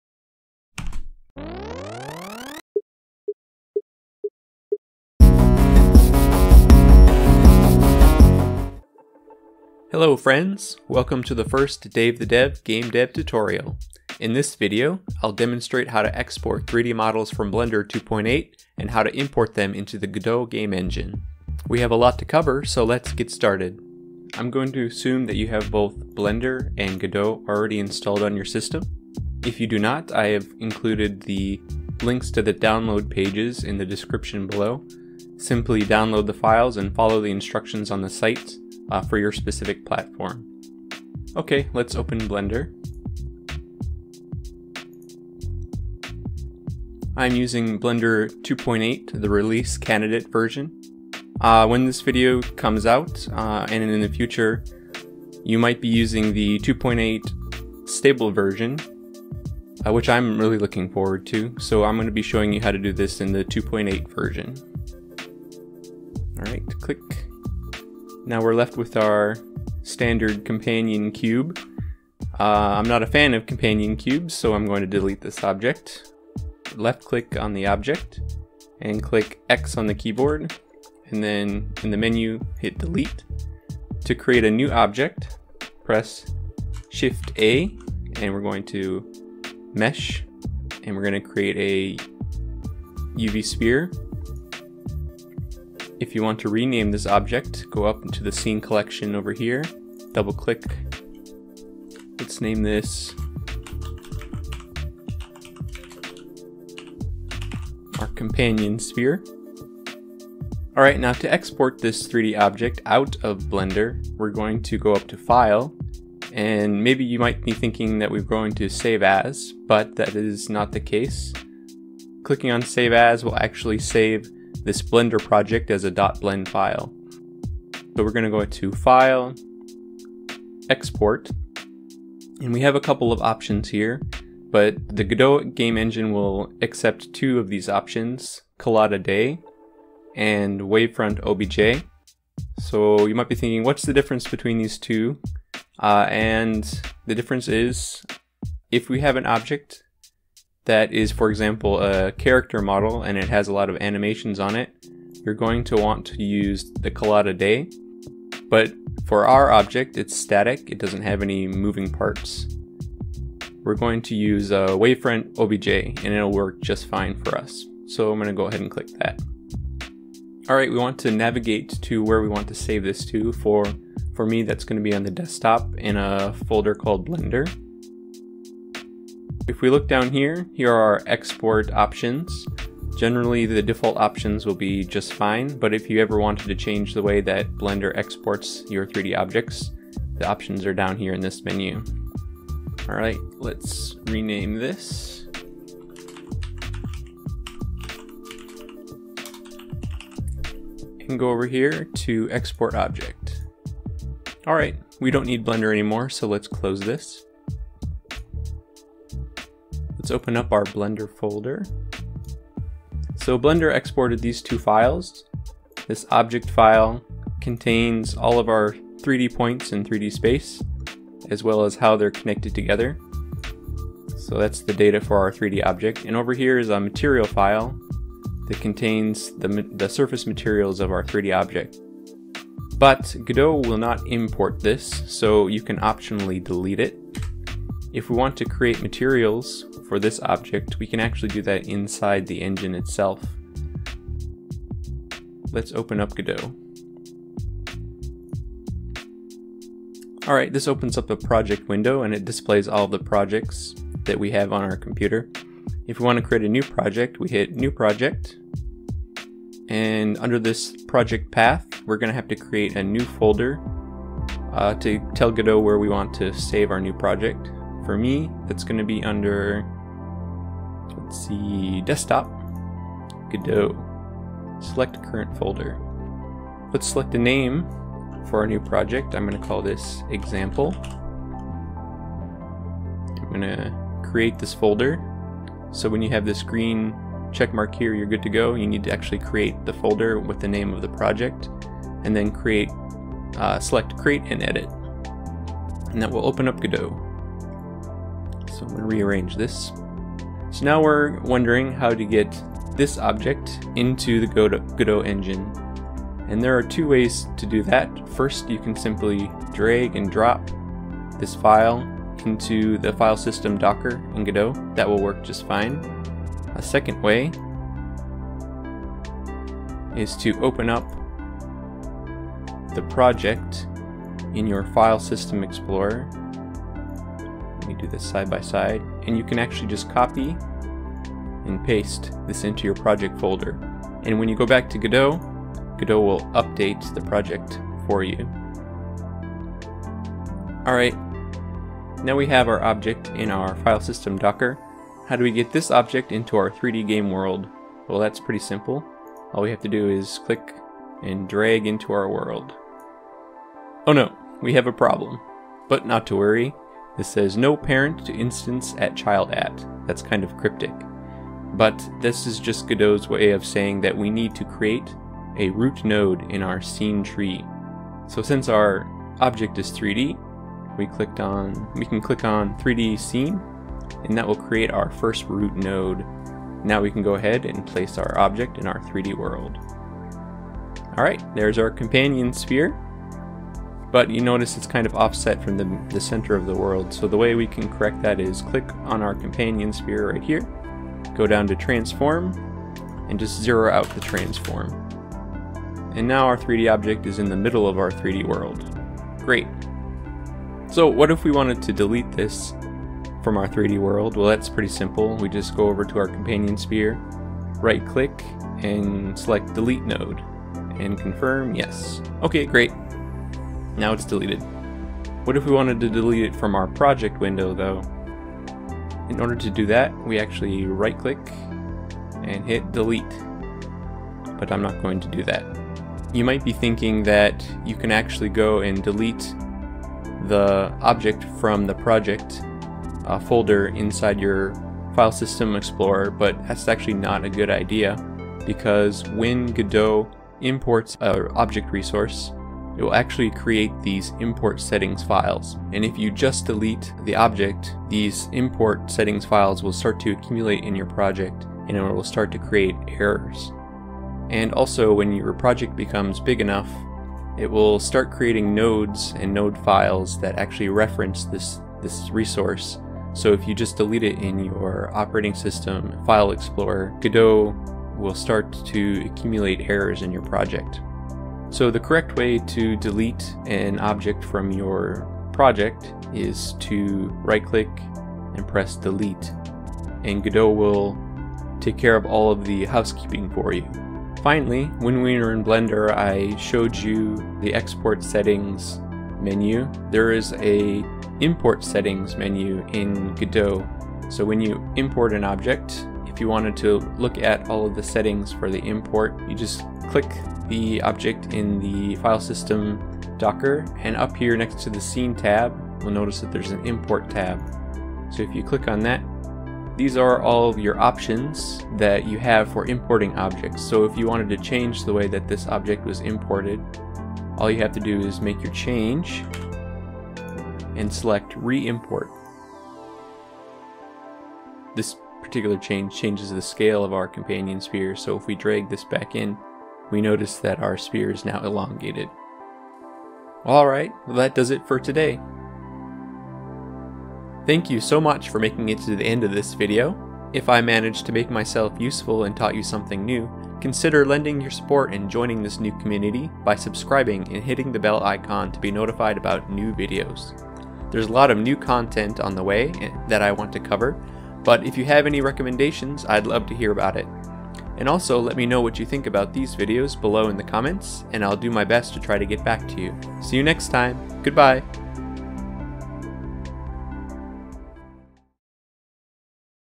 How to get 3D Models from Blender into Godot | Game Dev Tutorial 1
Welcome to the first video in the Game Dev Tutorial series. In this video I'll demonstrate how to export 3D models from Blender 2.8 and import them into the Godot Game Engine. I'd love to hear from you! If you have questions or recommendations please put them in the comments below and I'll do my best to answer.

Have fun! Happy Game Deving!